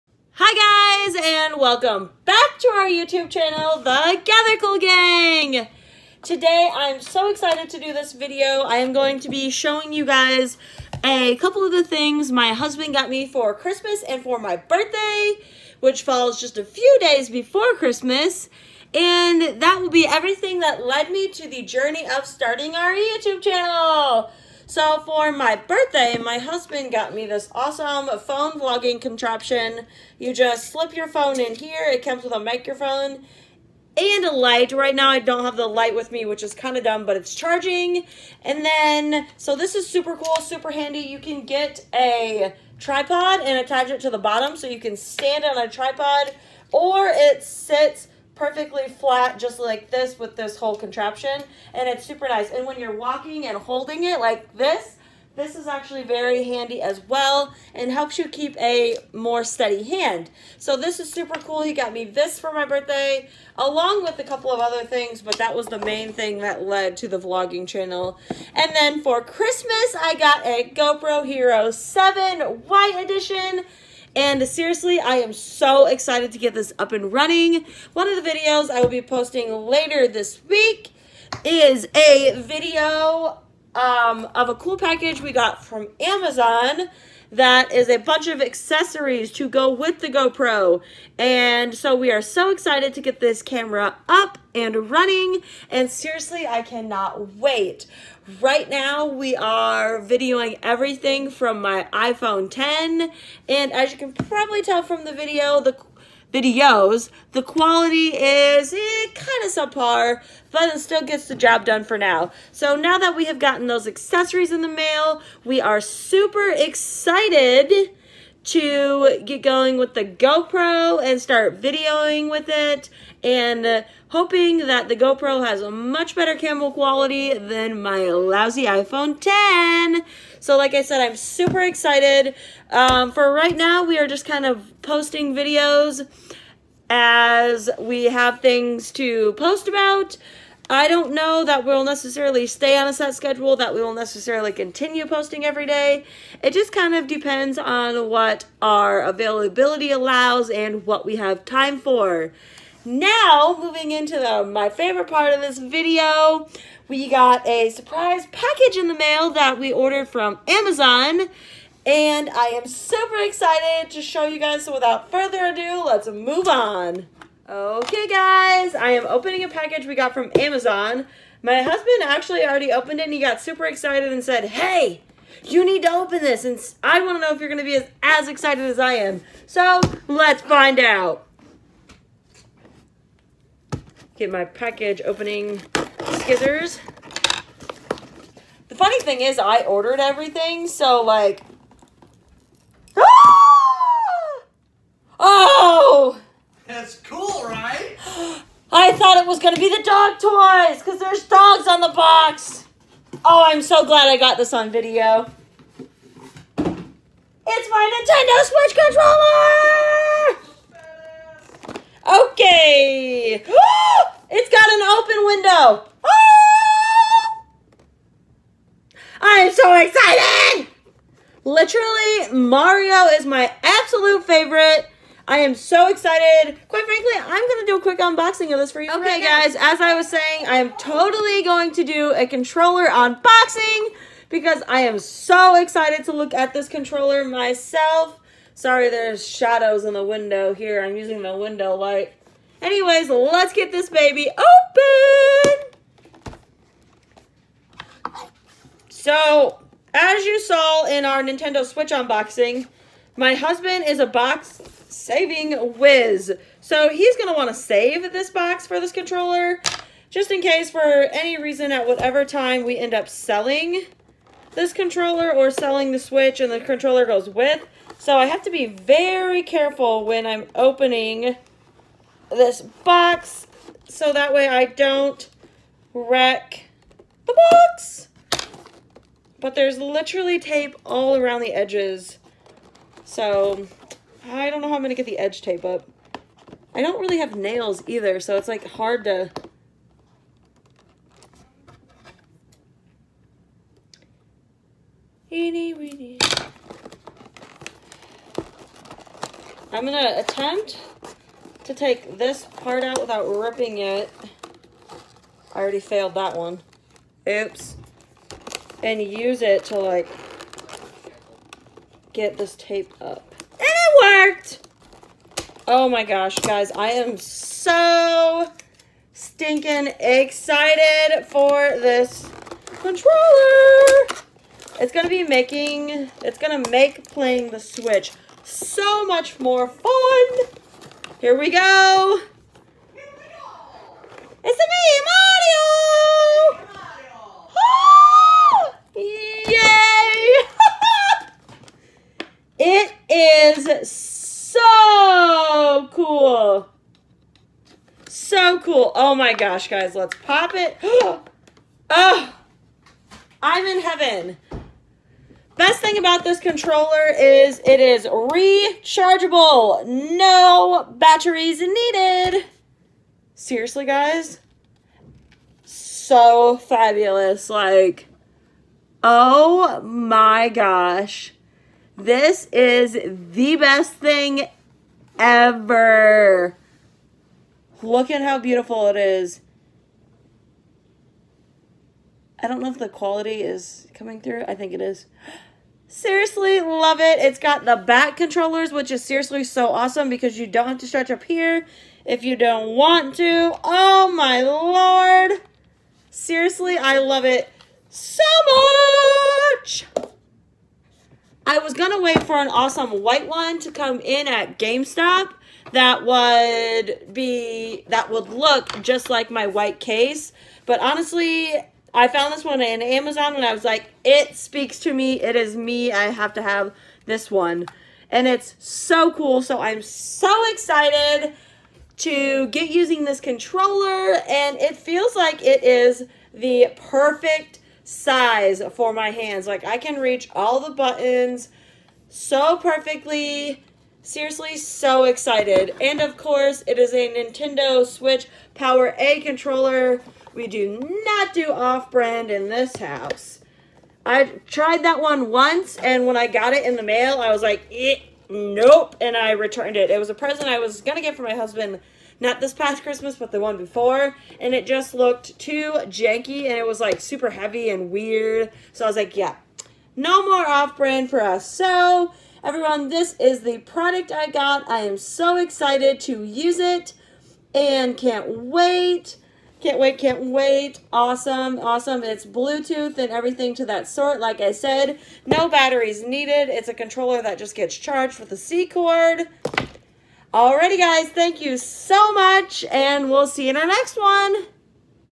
HE BOUGHT ME WHAT FOR CHRISTMAS??? Plus a Surprise package from amazon!!!
Super excited to show you what I got for Christmas!